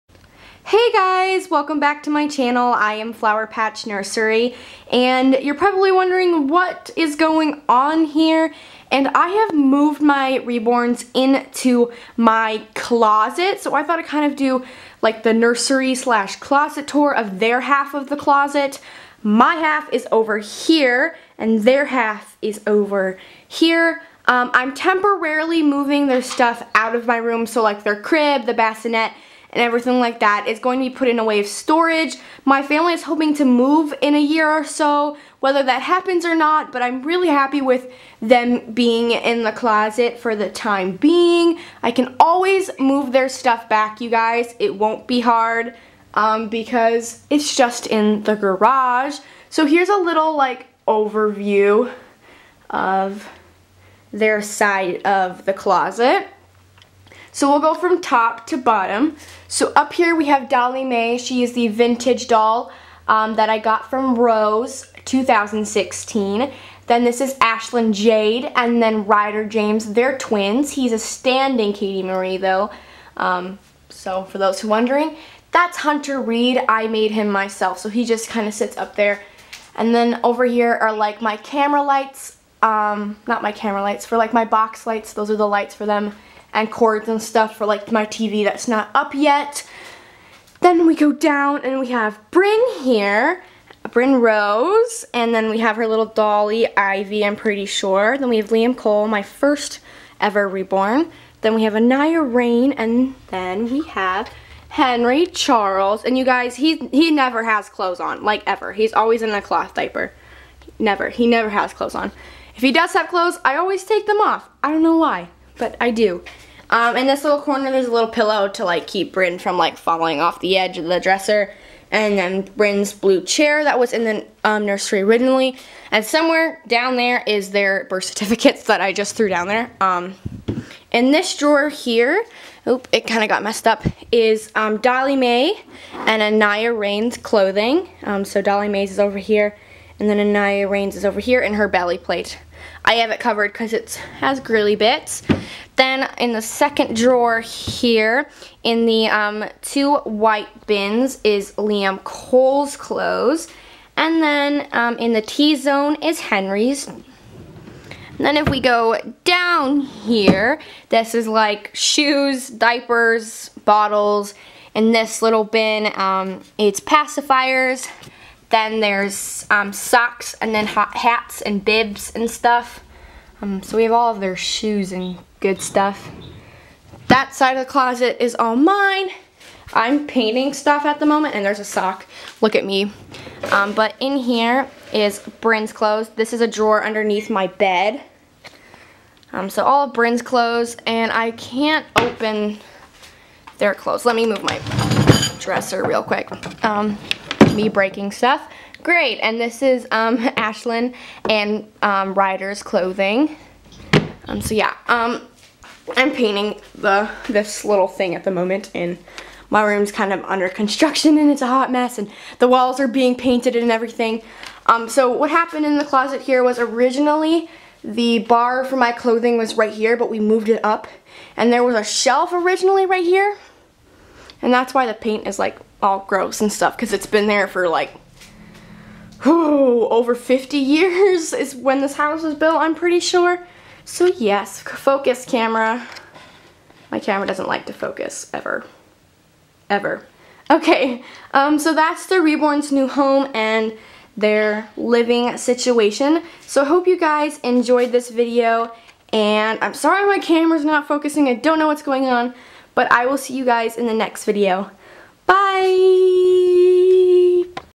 Reborn Nursery Tour
♕My collection ♕
Child doll ~ Brynn Rose
Toddlers ~ Ashlynn Jade & Rider James
Silicone ~ Henry Charles
Baby's ~ Liam Cole & Anaya Rain

☆About me☆
Who old are you ~ I'll be 15 in June
What's your name ~ Lucy
Where are you from ~ U.S